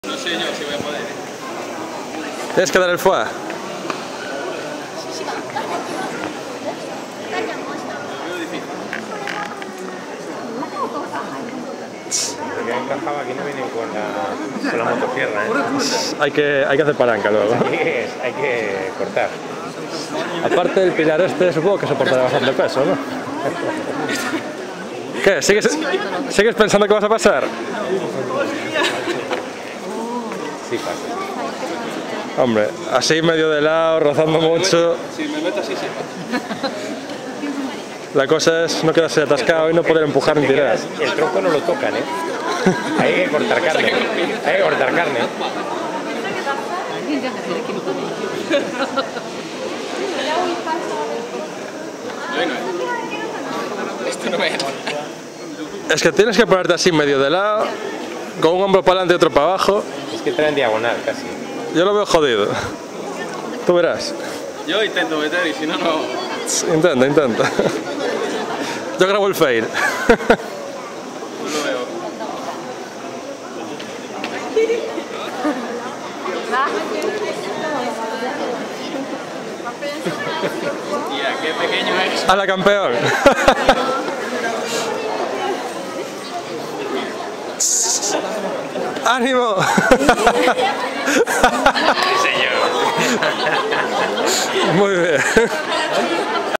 Agujero del pilar del templo de Todai-ji, en Nara
Uno de los pilares del Todai-ji (un templo que es el edificio de madera más grande del mundo) tiene un agujero del tamaño de un orificio nasal del buda gigante del mismo templo. Se dice que quien lo atraviesa alcanzará la iluminación.